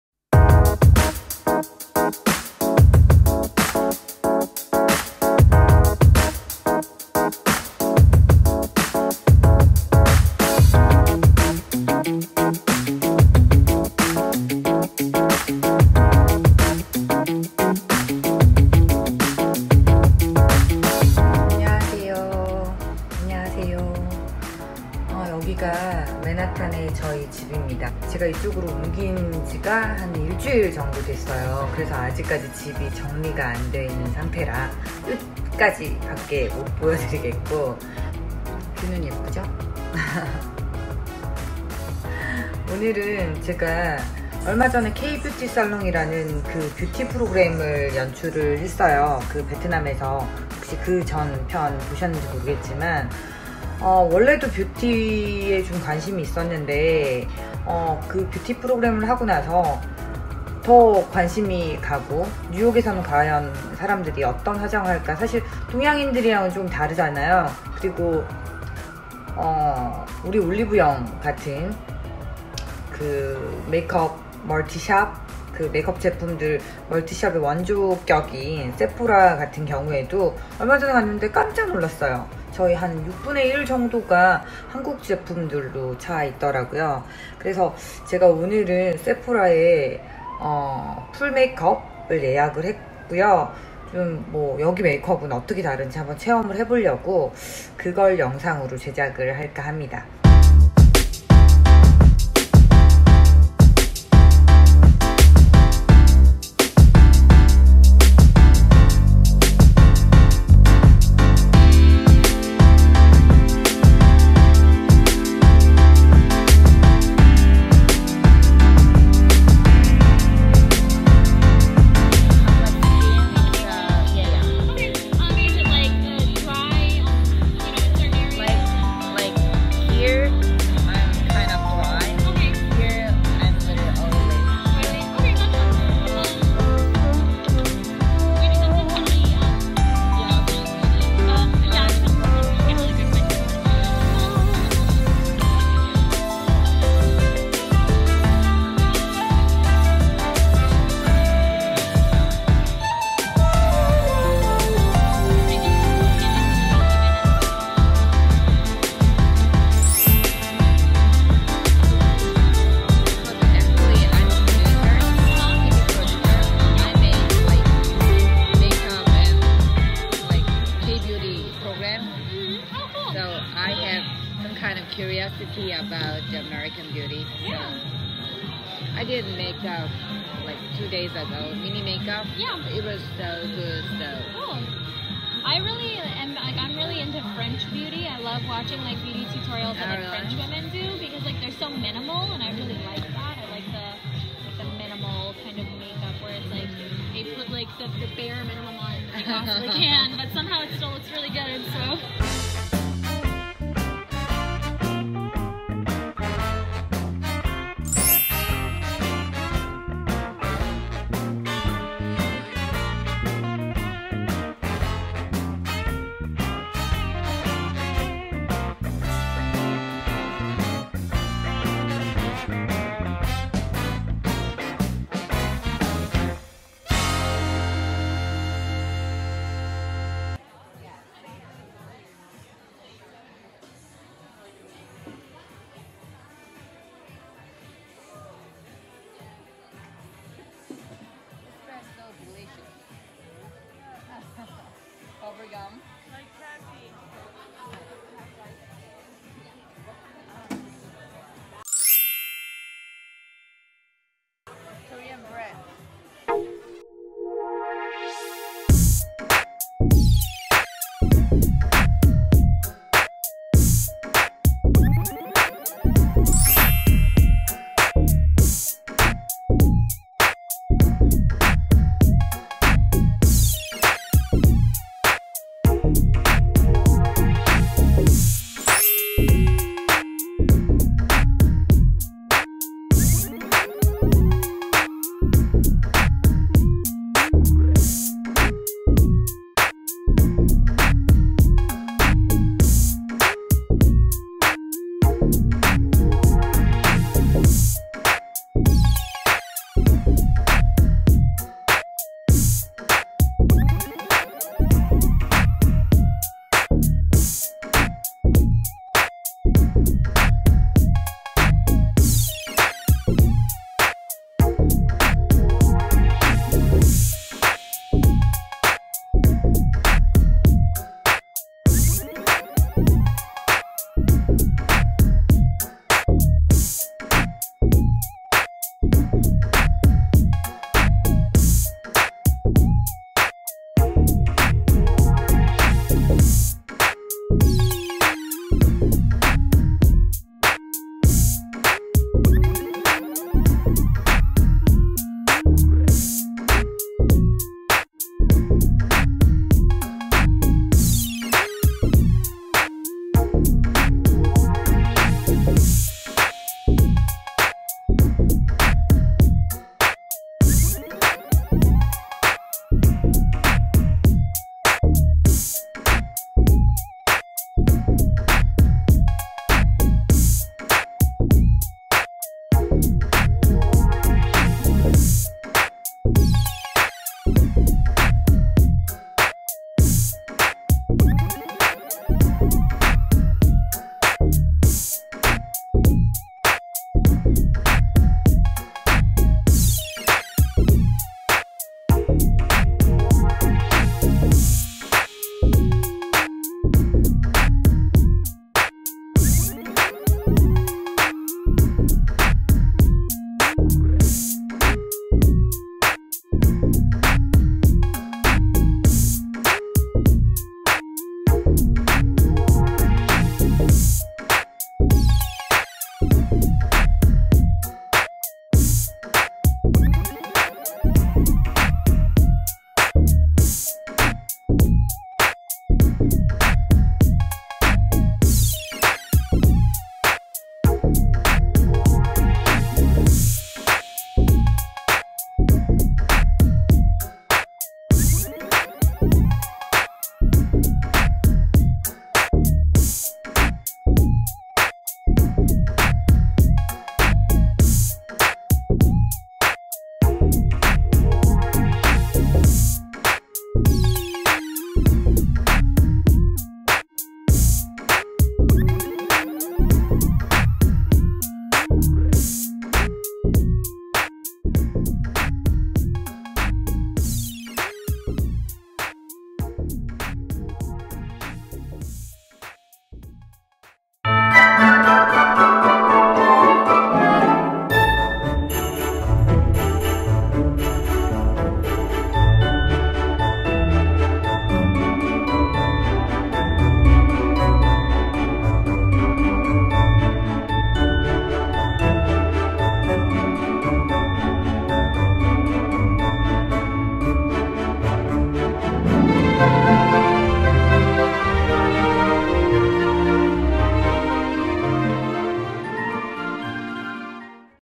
[PD Chloe] NYC Makeup Sephora 체험기
**Please hit the CC button and choose ur language✌🏼
**자막을 보실 수 있도록 넣어두었습니다. 자막버튼을 눌러 주세요^👀👀

미국메이크업 vs 한국메이크업

어떻게 다를까??!!
PD Chloe의 미국메이크업 체험기

입체적인 화장을 강조하는 미국메이크업
피부톤을 강조하는 한국메이크업

전격 비교 공개^^